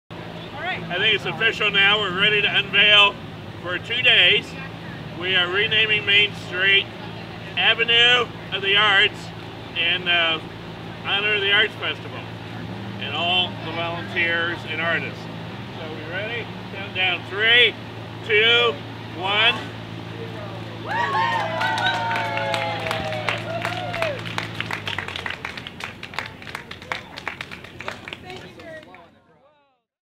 20th Carmel International Arts Festival - Avenue of the Arts
On the fourth weekend in September, Main Street in Carmel Indiana is renamed to Avenue of the Arts for the Carmel International Arts Festival.

This was the inaugural dedication of the Avenue of the Arts in 2017.